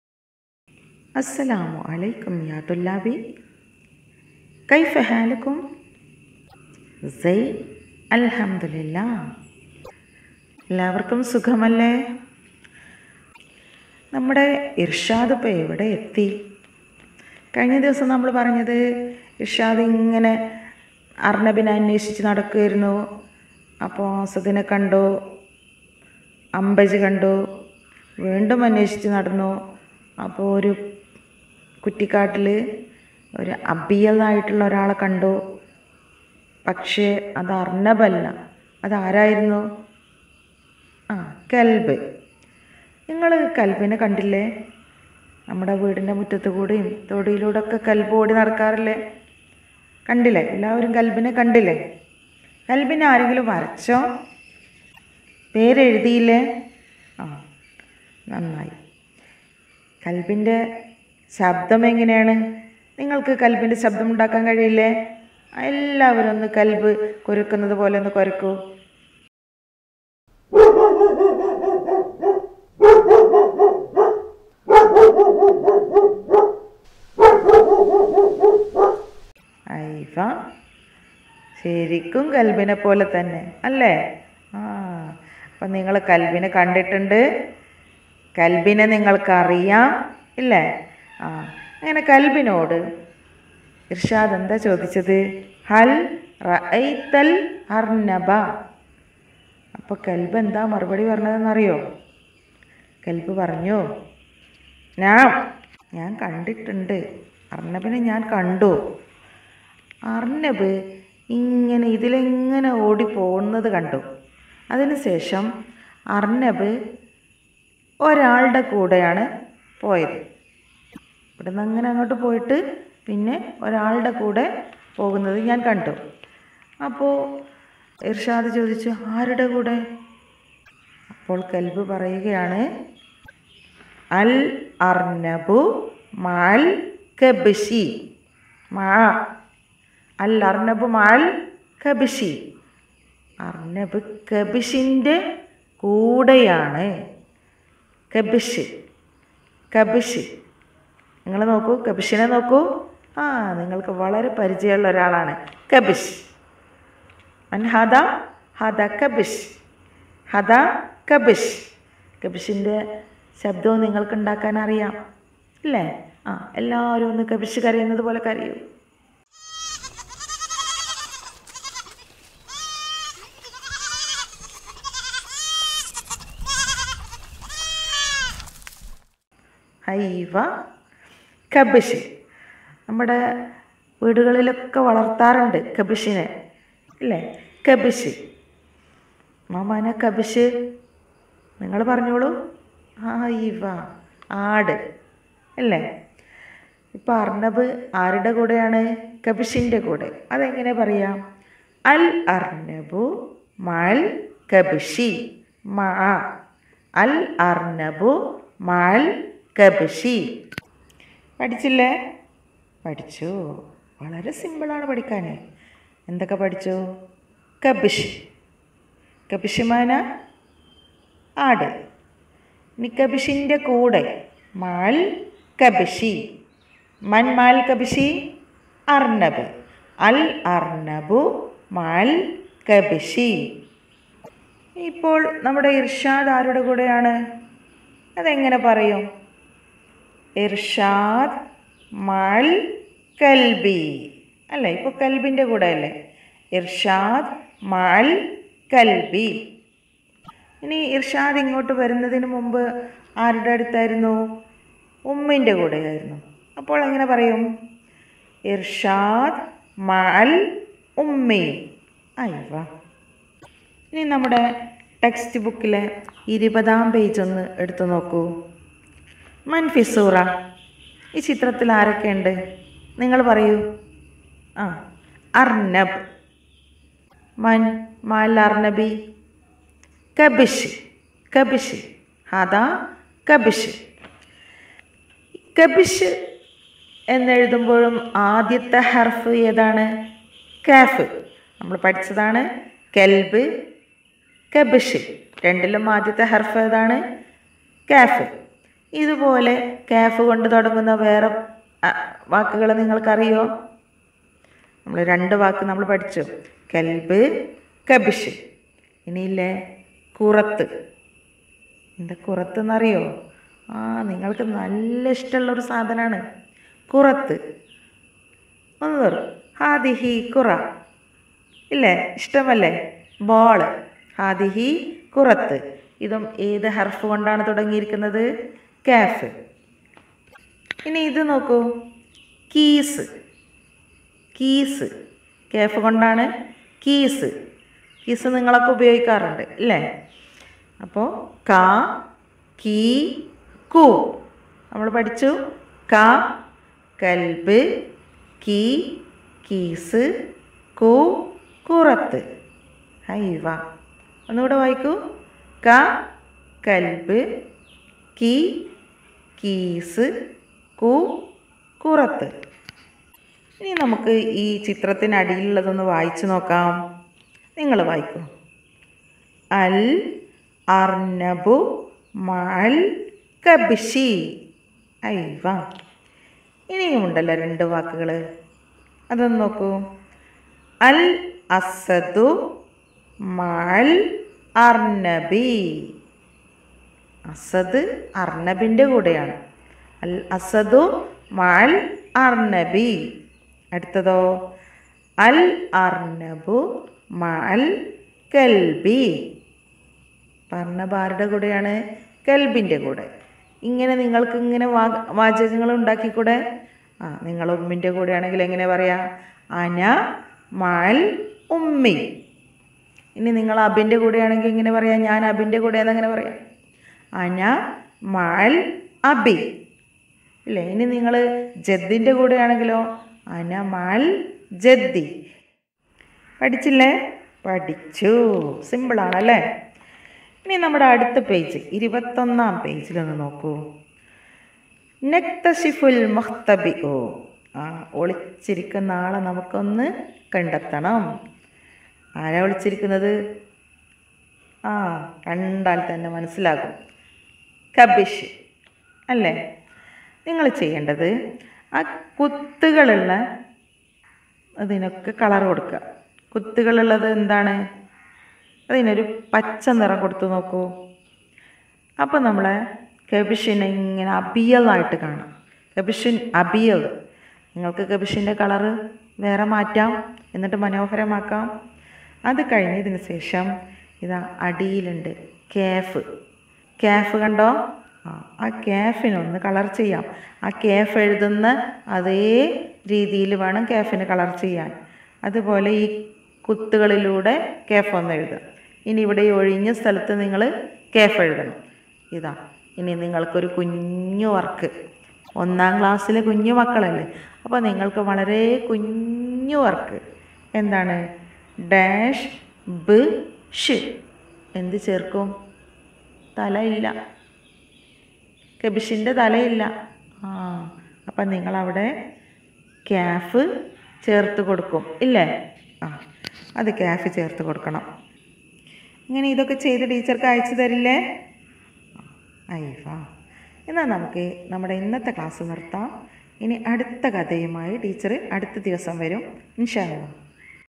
Class 1 | Unit 1| Page 20,21 (Part - 7) | Arabic online class | Kerala Arabic Reader
Online class for 1st std students of Kerala state syllabus.

Presentation - Balkees T K
Edits - Fifi C
          -Juby sameera T K

ക്ലാസുകൾ ലഭിക്കുവാൻ താഴെയുള്ള ലിങ്കുകളിൽ ക്ലിക്ക് ചെയ്യുക.

മൂന്നാം ക്ലാസ്

നാലാം ക്ലാസ്